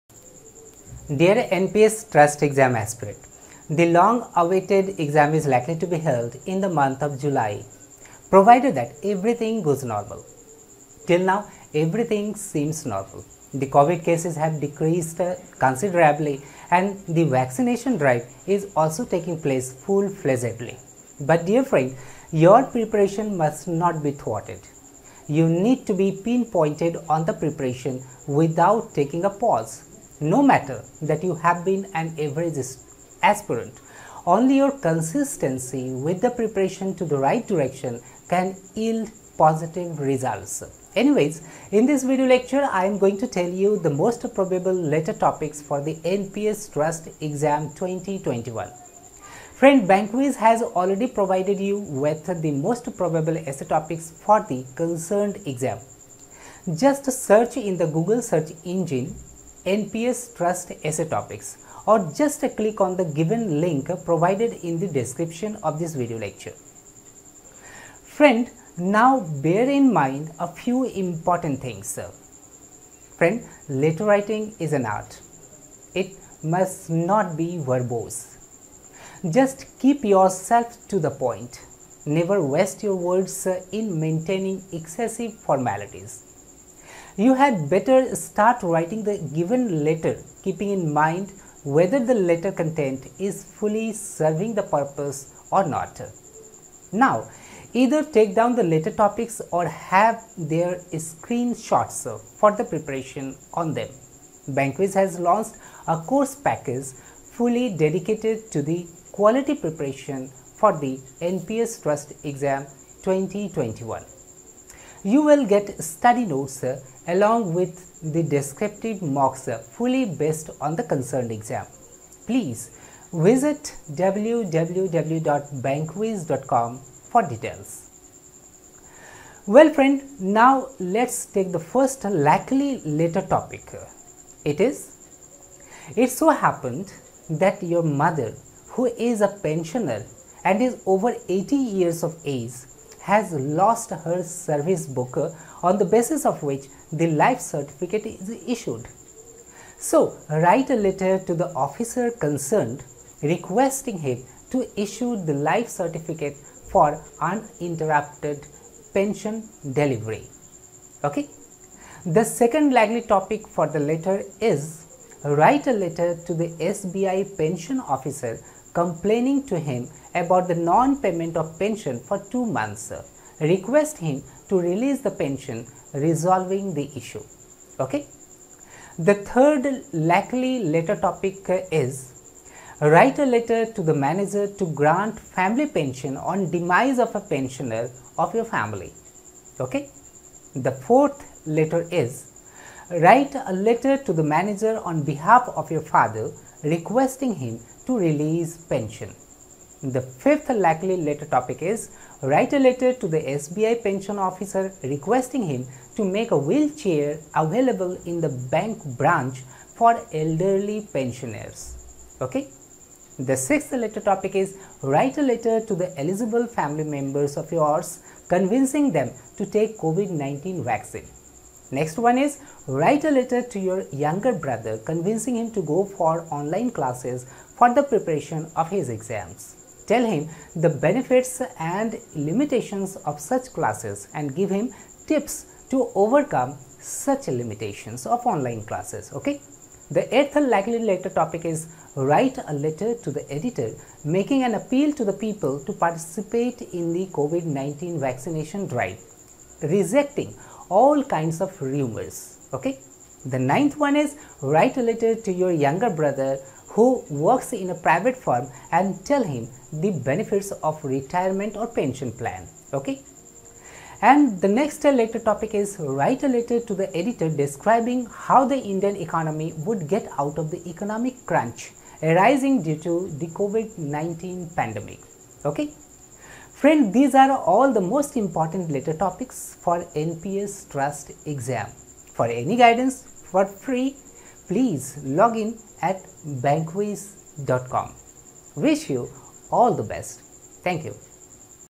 Most Probable Letter Topics for NPS TRUST Officer Grade B Manager 2021
Letter-writing is an art. It must not be verbose. Just keep yourself to the point. Never waste your words in maintaining excessive formalities. You’d better start writing the given letter asking yourself whether the letter content is fully serving the purpose or not.